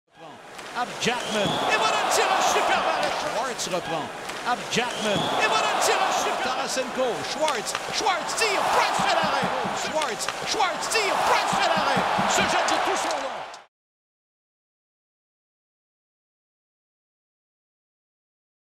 Deux fois plutôt qu'une / Twice as nice
Carey Price réussit deux arrêts spectaculaires en moins de 30 secondes pour garder son équipe en avance en troisième période.

Carey Price makes a pair of huge saves early in the third to keep the Habs' one-goal lead intact.

2014/11/20 vs. STL